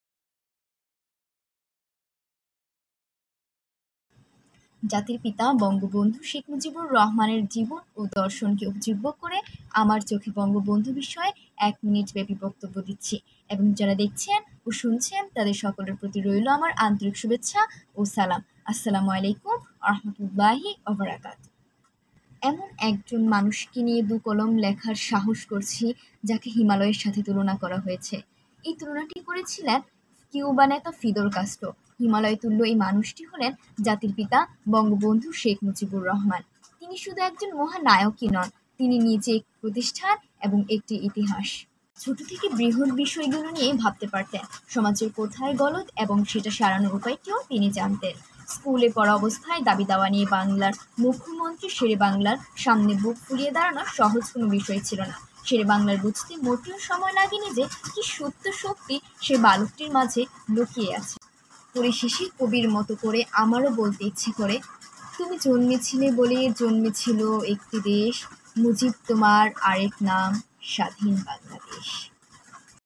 আমার চোখে বঙ্গবন্ধু - উপস্থাপনায়- সাবিকুন ইসলাম ইনতা- ৯ম শ্রেণি BCGHS- Haziganj,Chandpur.
আমার চোখে বঙ্গবন্ধু